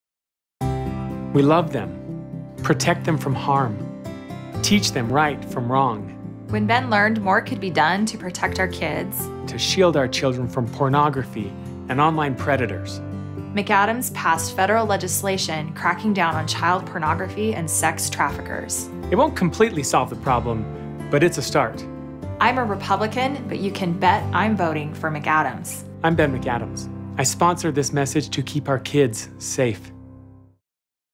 Protect
Ben McAdams passed legislation to protect our children on the internet and to crack down on child pornography and sex trafficking.